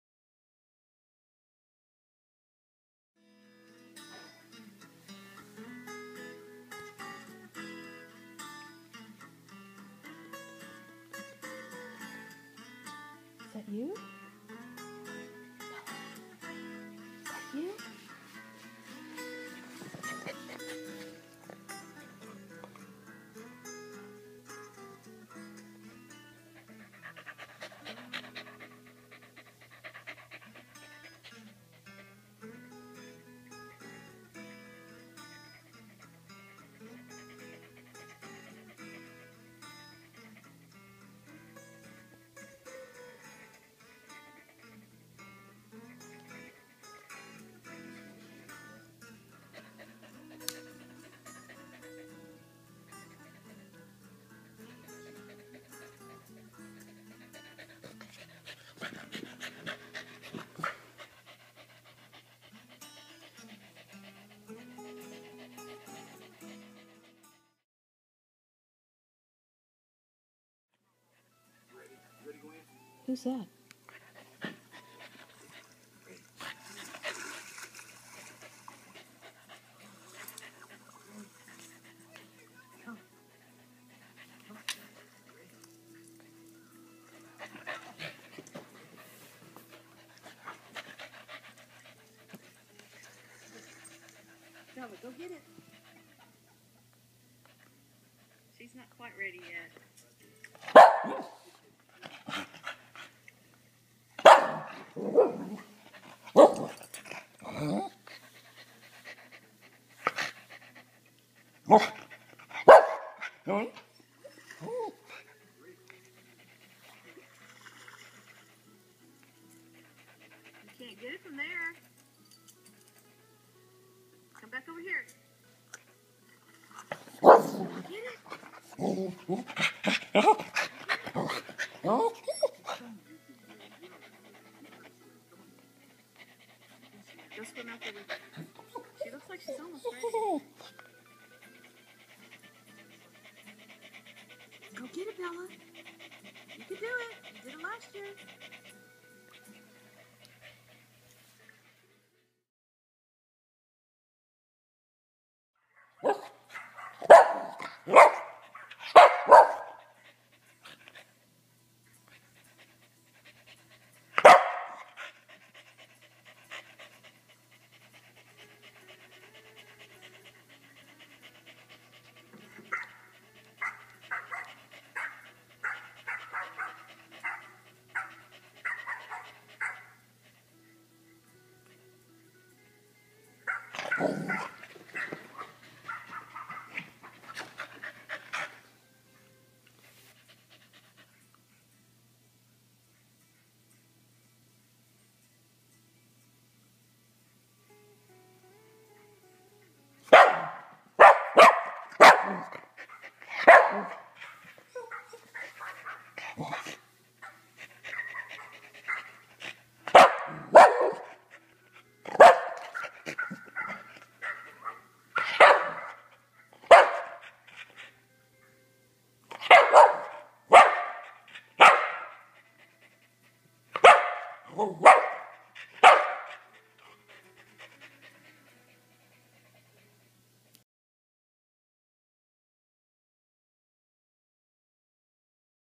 Bella Watches Herself on Video
I was editing some videos of my fur baby Bella and she got up and started to watch herself...these are some of her reactions.